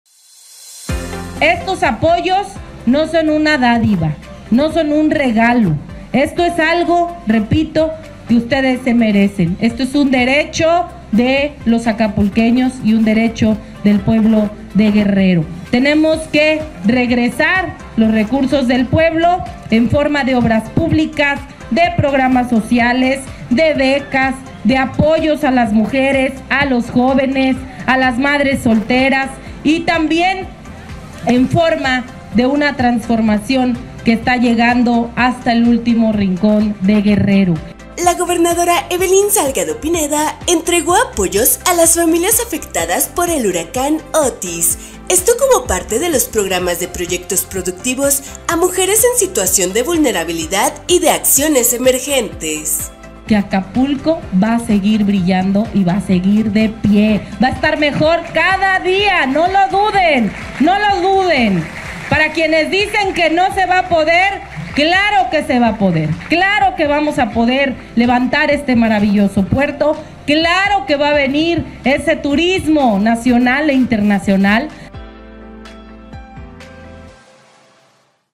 Entrega Gobernadora apoyos a las familias afectadas por el huracán Otis
La gobernadora Evelyn Salgado Pineda entregó apoyos a las familias afectadas por el huracán Otis, esto como parte de los programas de Proyectos Productivos a Mujeres en Situación de Vulnerabilidad y de Acciones Emergentes, en lo que se invirtieron poco más de 3.2 mdp para 108 proyectos y mil 83 tinacos.